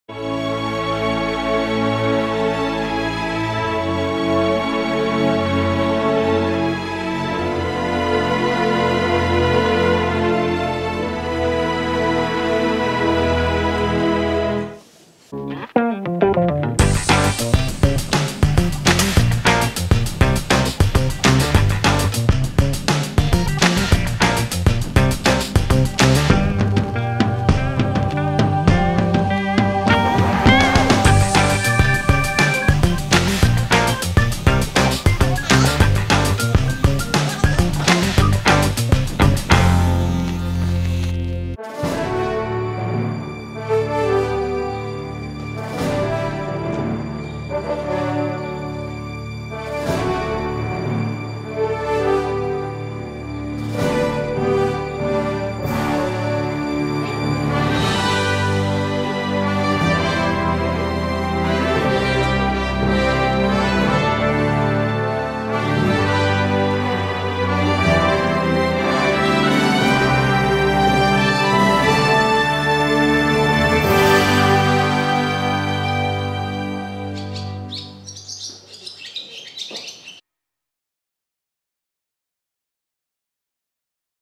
Foal clipping metamorphosis
American miniature colt "MagicEquus Mellow Beauty" gets his first clipping and turns from teddy bear into show horse. Mellow just loved getting clipped, just look at his face enjoying it!